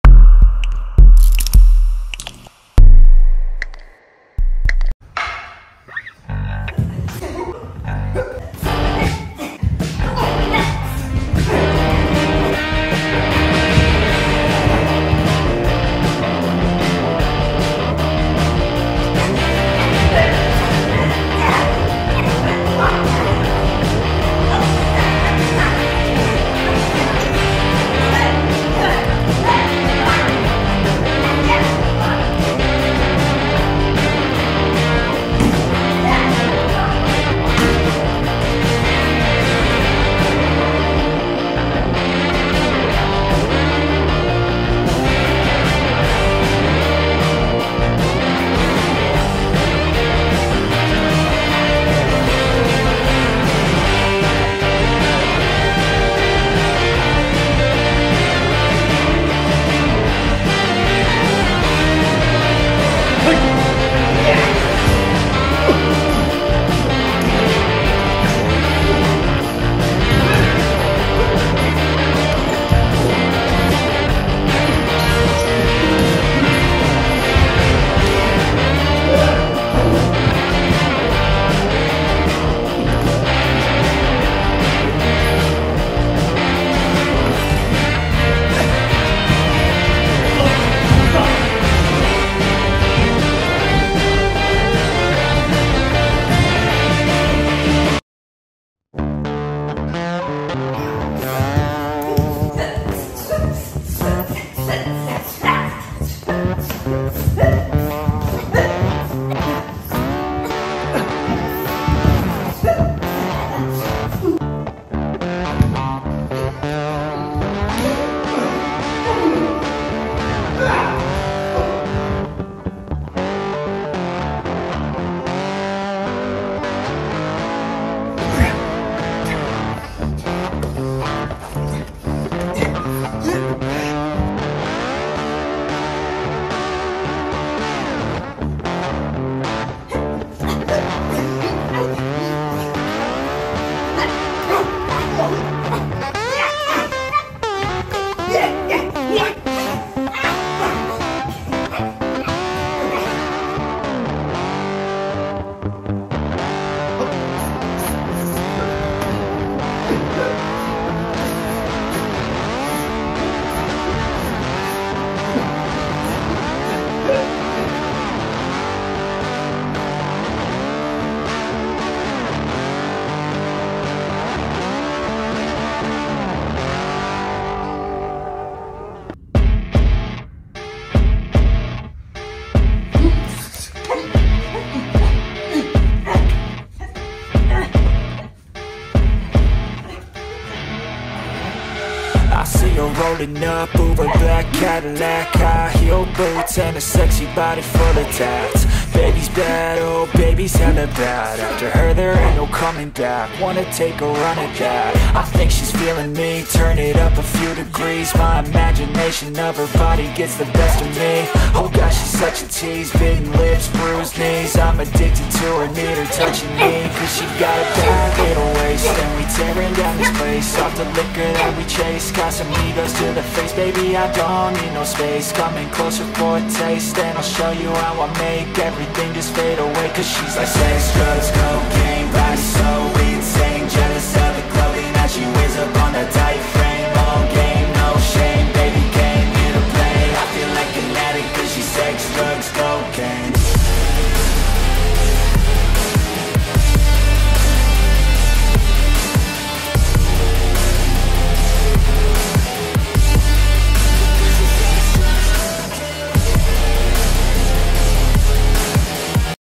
リアルアクション稽古
格闘アクション映画の為の稽古
リアルに稽古ガチアクション